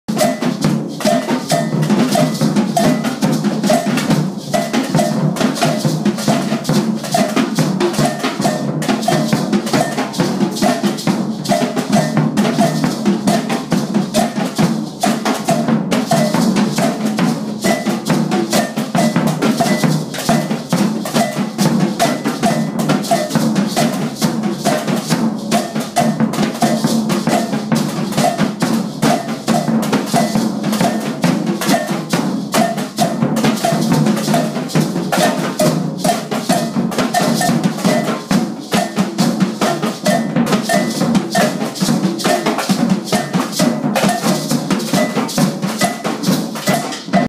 Florida Keys Community College African Drumming Performance
This is a group of students in a Music Appreciation class at Florida Keys Community College performing parts of the traditional Ghanaian celebration Drum Gahu. They learned to read the rhythms and play the instruments in 5 hours.